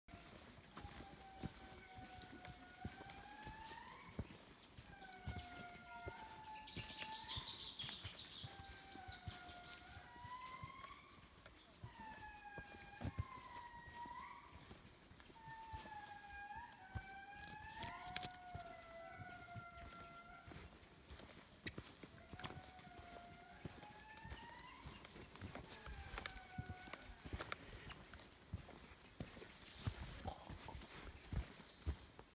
Andasibe NP walk Madagascar
It was amazing walk in the rain forest the Andasibe NP In Madagascar